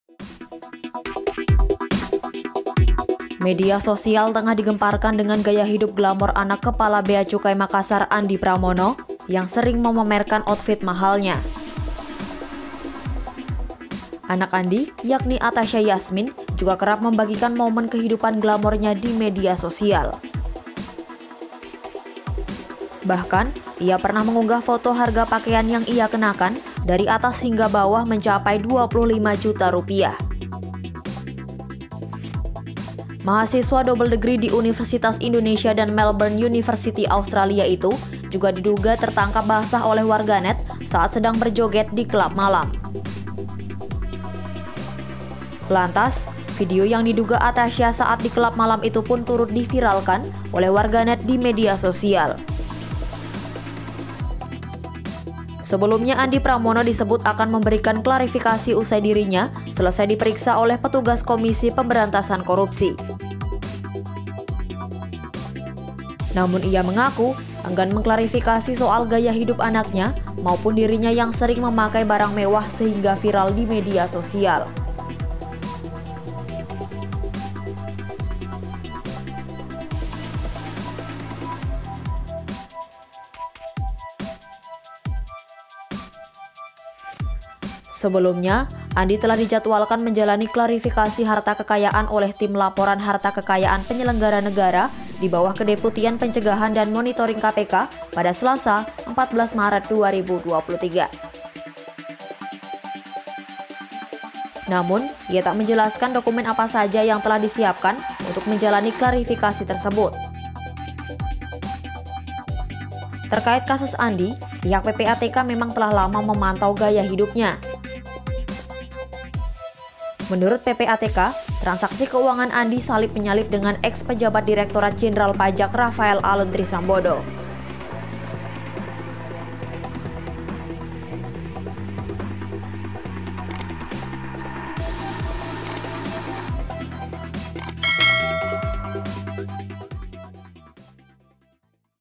Anak Kepala Bea Cukai Makassar Kerap Pamer Gaya Hidup, Harga Baju Rp 25 Juta
Bahkan pada salah satu unggahan fotonya harga pakaian yang Ia kenakan dari atas hingga bawah mencapai Rp 25 juta.

Mahasiswa double degree di Universitas Indonesia dan Melbourne University, Australia itu juga diduga tertangkap basah oleh warganet saat sedang berjoget di kelab malam.

Video Jurnalis: TAL
Penulis: Syakirun Niam
Penulis Naskah: Dariz Kartika
Narator: Dariz Kartika
Video Editor: Dariz Kartika
Produser: Elizabeth Prillia Yahya Carvallo